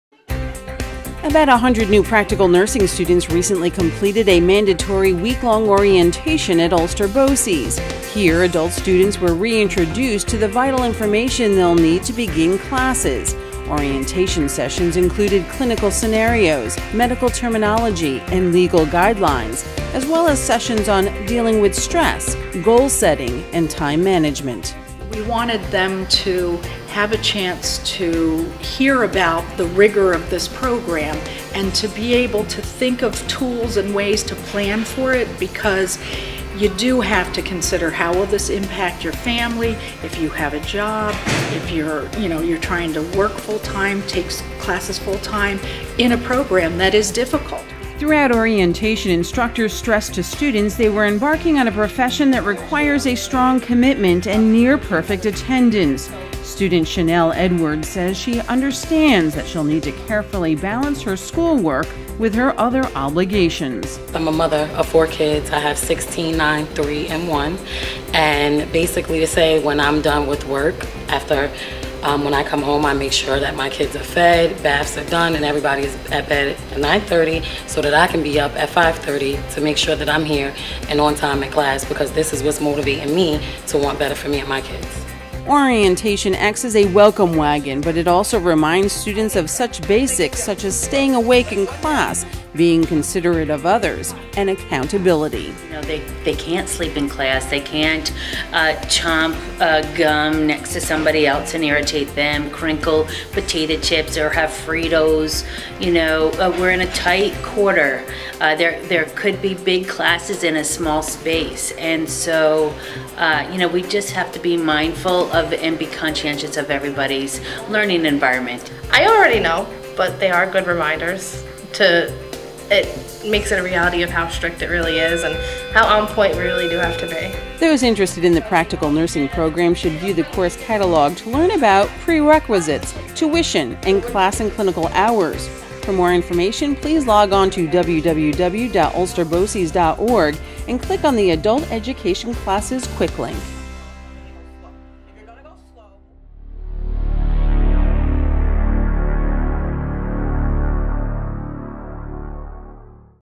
Nursing Program Orientation
Congratulations, you decided to sign up for the Practical Nursing Program at Ulster BOCES. There is a lot of information to learn. But before you get too involved in your studies, you have to get through orientation week. Here's a sneak peek at what to expect.